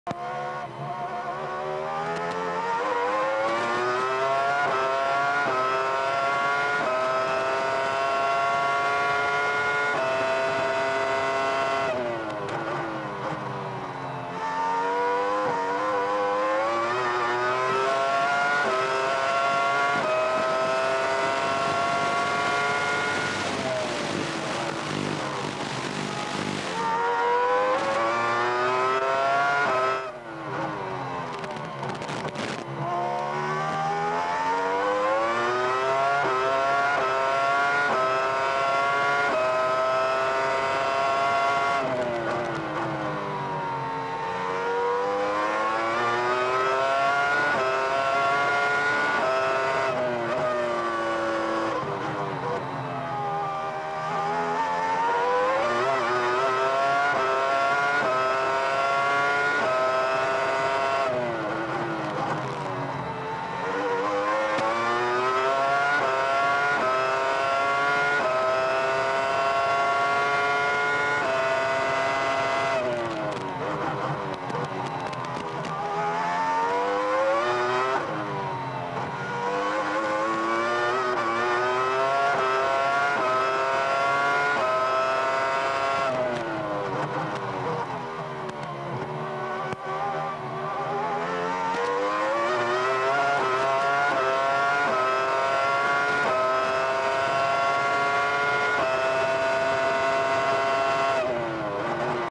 F1 Imola 2000 - Michael Schumacher Onboard
Enjoy some more onboard action with the Ferrari of German driver Michael Schumacher during practice for the 2000 San Marino Grand Prix at the Autodromo Enzo e Dino Ferrari circuit. Also in my opinion, the 2000 Ferrari car was the best sounding one in Formula 1.